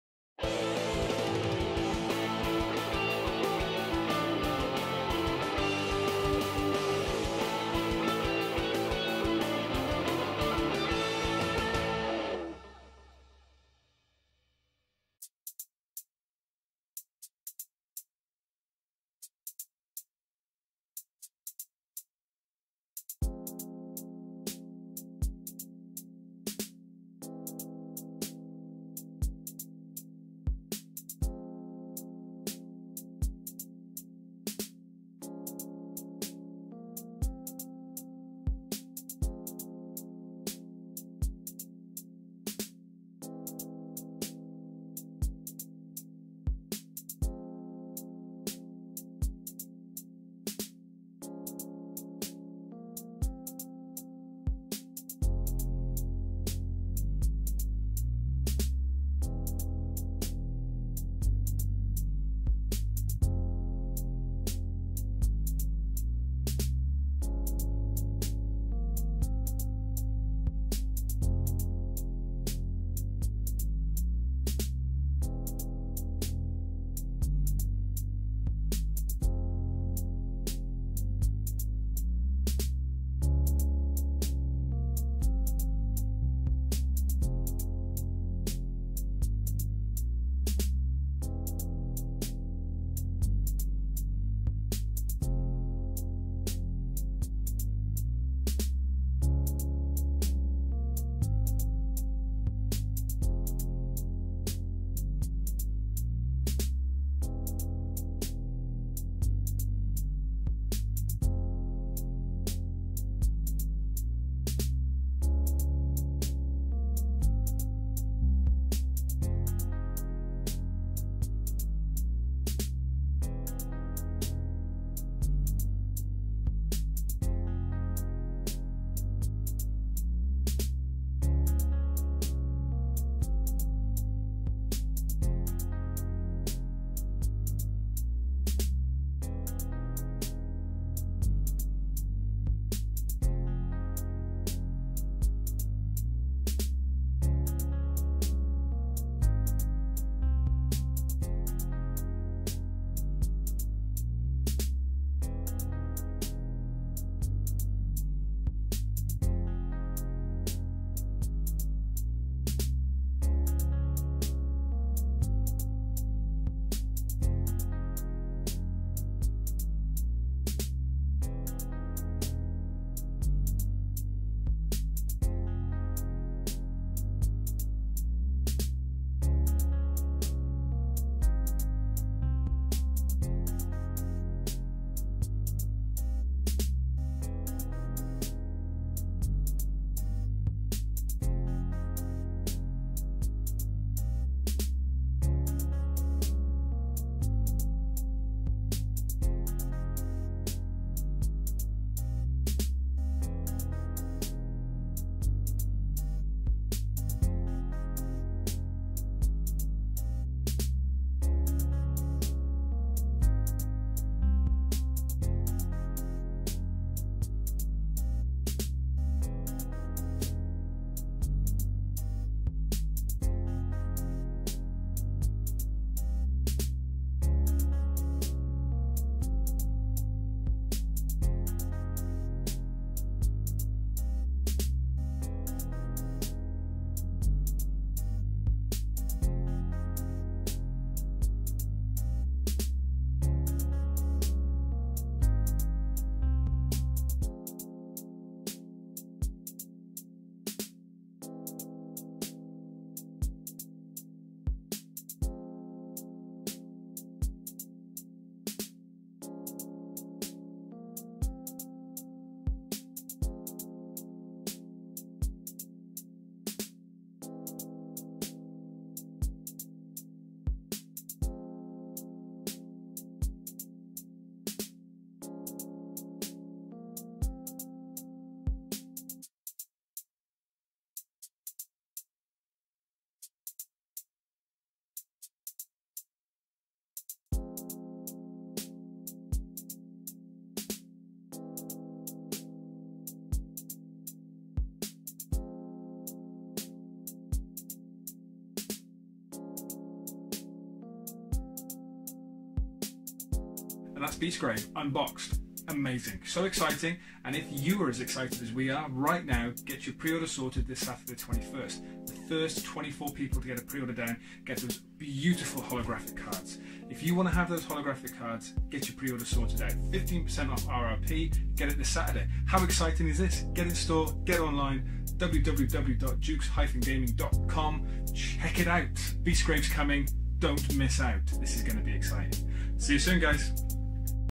Unboxing: Warhammer Underworlds - Beastgrave
Duke's Gaming
Join us as we open up the newest season of Warhammer Underworlds from Games Workshop, Beastgrave!

We'll be bringing more unboxinngs, game plays, painting tutorials